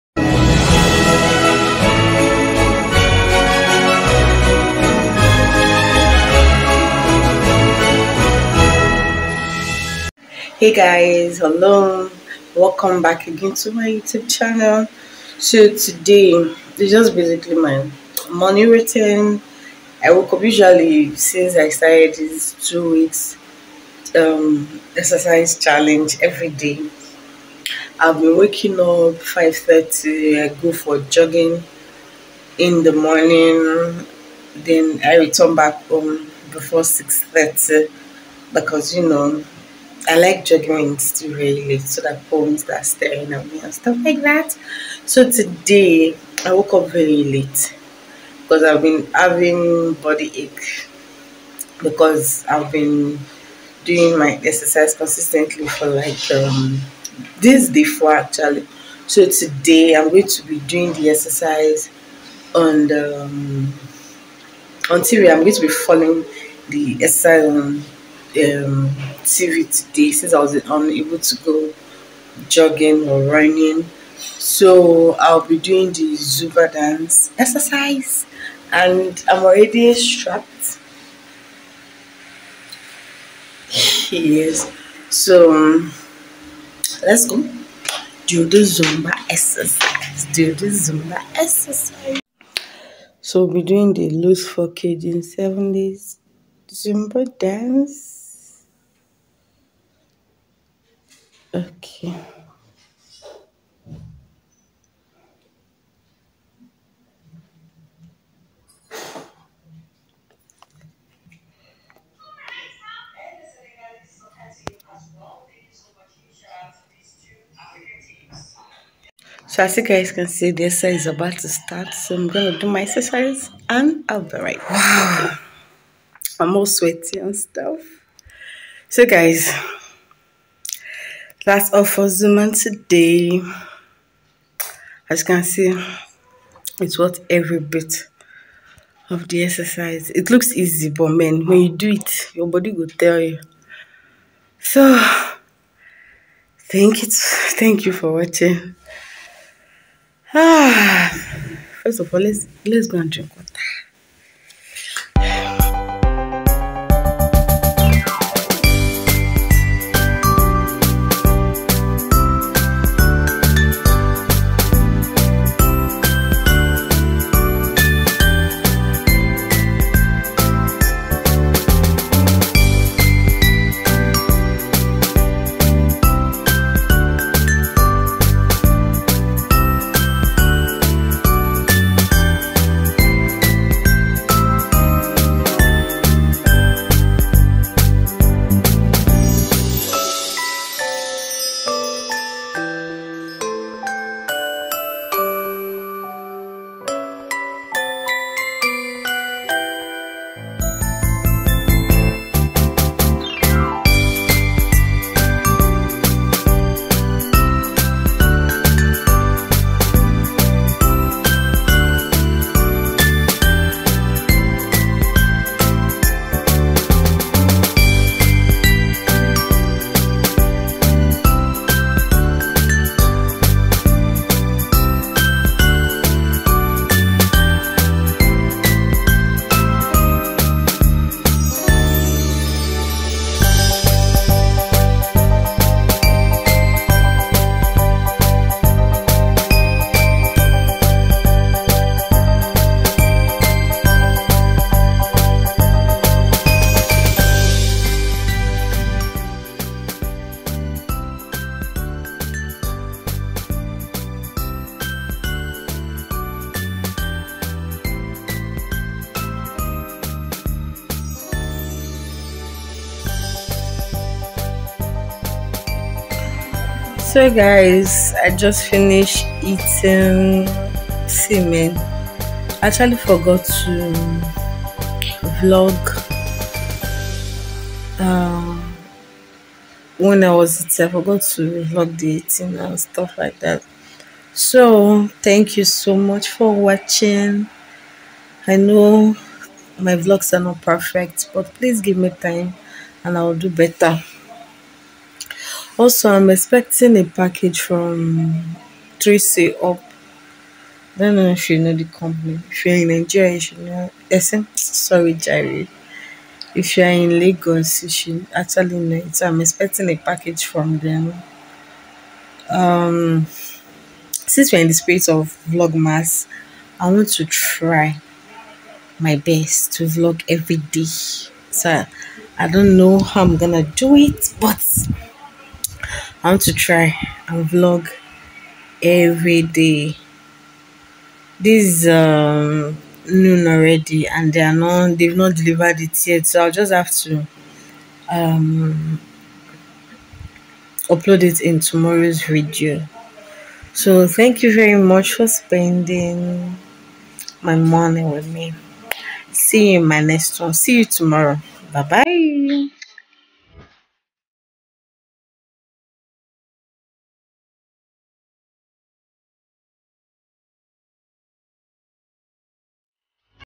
vlogmas+14days weightloss challenge+morning routine
My morning routine since i started my 2weeks weightloss challenge